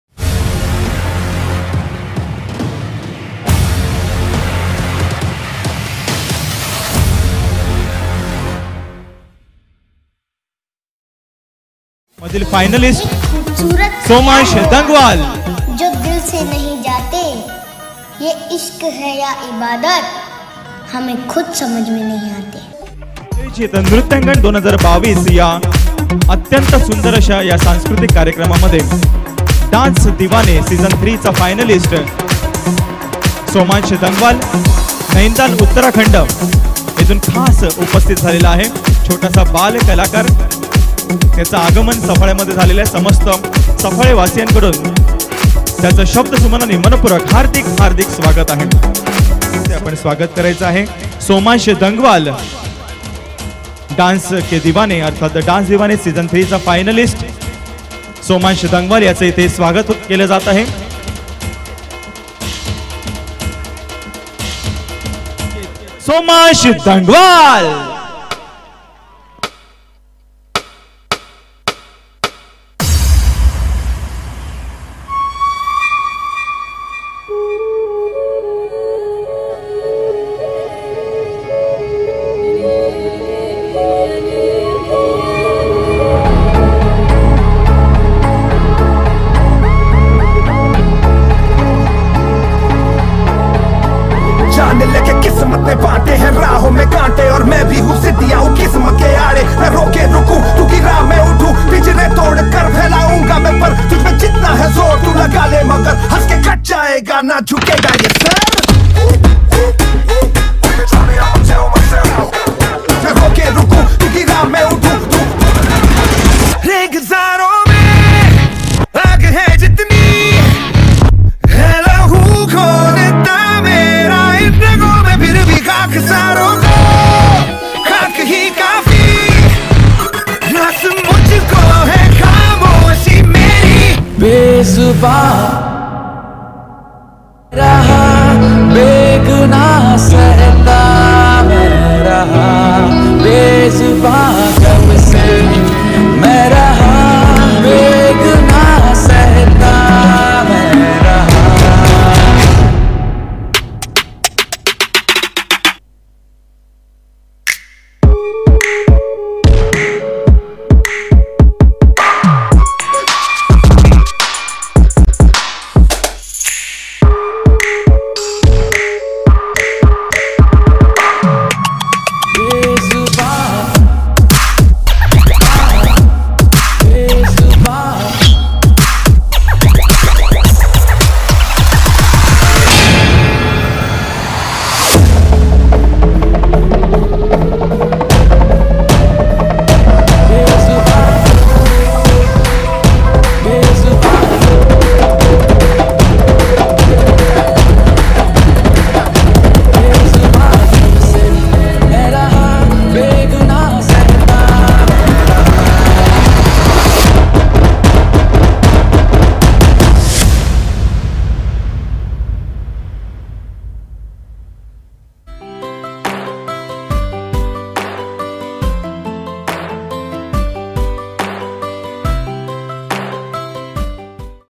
SOMANSH DANGWAL | SPECIAL GUEST PERFORMANCE | NRUTYANGAN 2022 | CREATIVE DANCE ACADEMY
NRUTYANGAN IS A DANCE EVENT WHICH IS ORGANISE BY CREATIVE DANCE ACADEMY.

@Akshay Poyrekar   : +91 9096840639